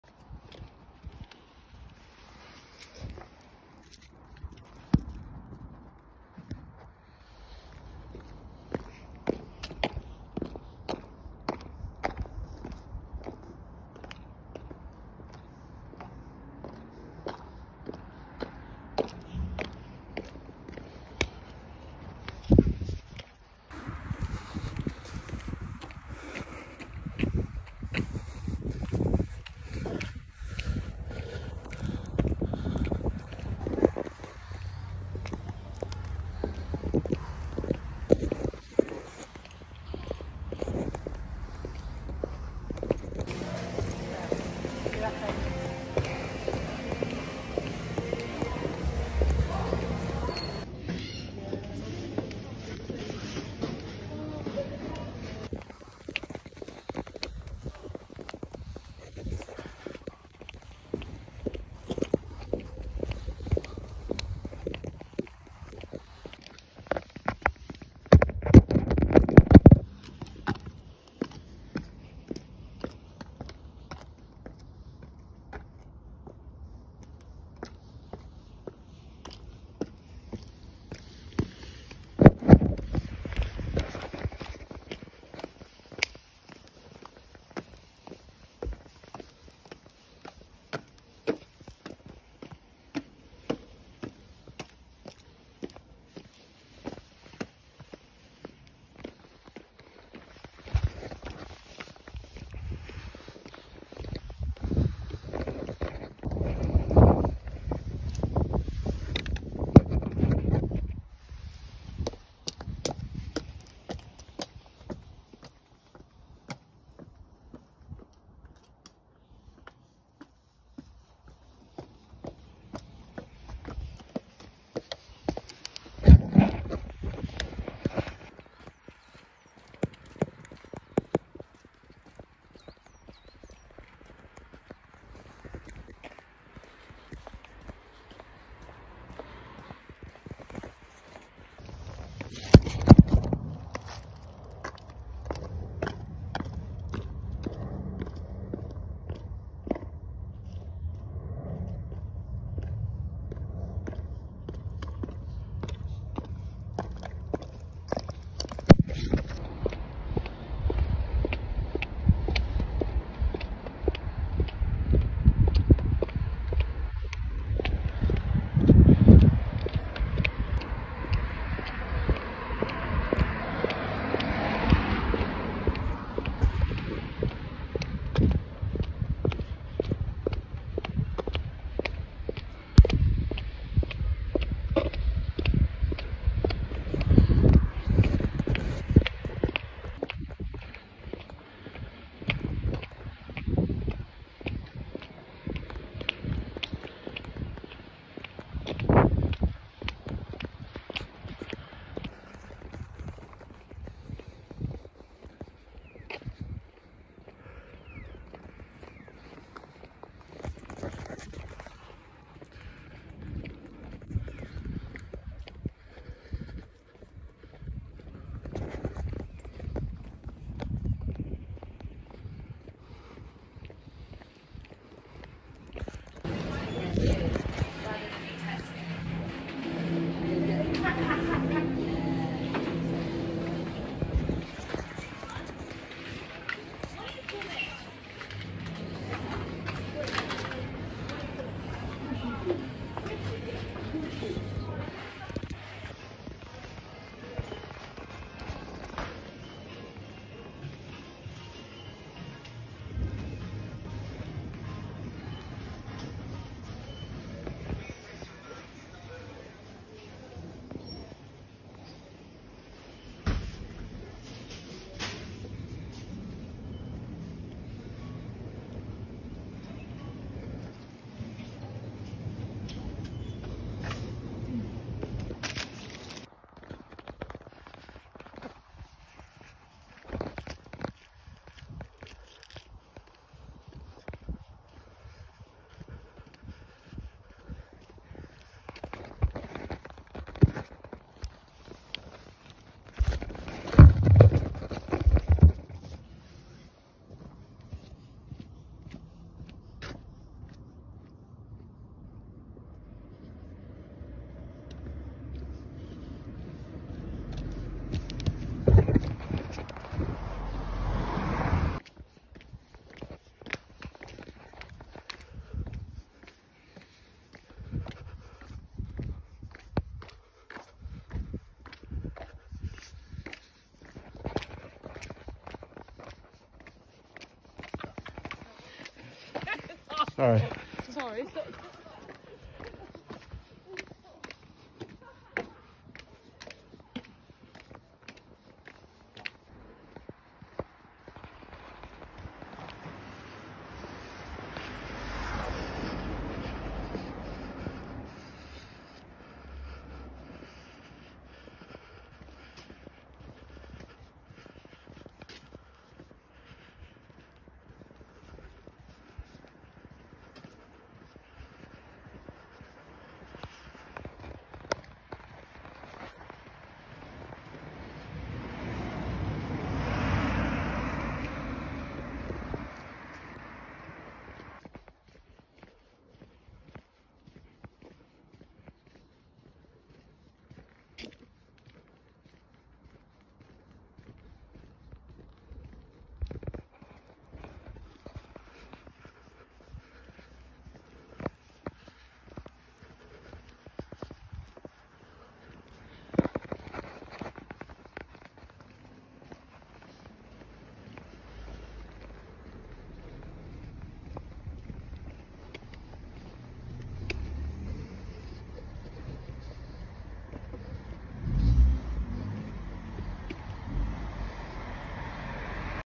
Crossdresser in public compilation
A compilation of me in public. Includes daytime, nighttime, quiet and busy places.